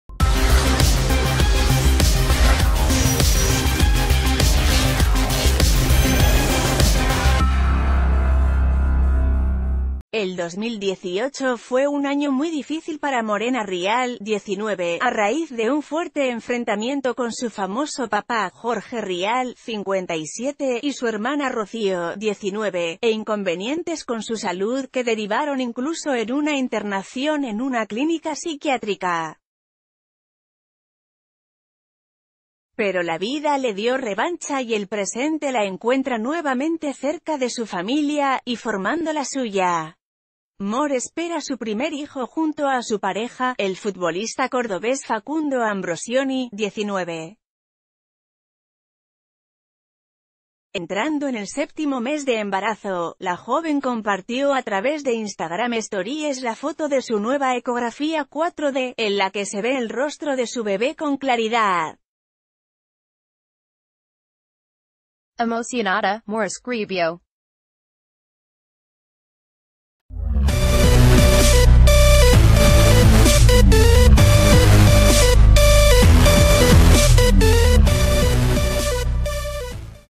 Morena Rial compartió la eco de su bebé junto a un conmovedor mensaje: "Gracias por salvarme"
El 2018 fue un año muy difícil para Morena Rial (19) a raíz de un fuerte enfrentamiento con su famoso papá, Jorge Rial (57) y su hermana Rocío (19), e inconvenientes con su salud que derivaron incluso en una internación en una clínica psiquiátrica. Pero la vida le dio revancha y el presente la encue...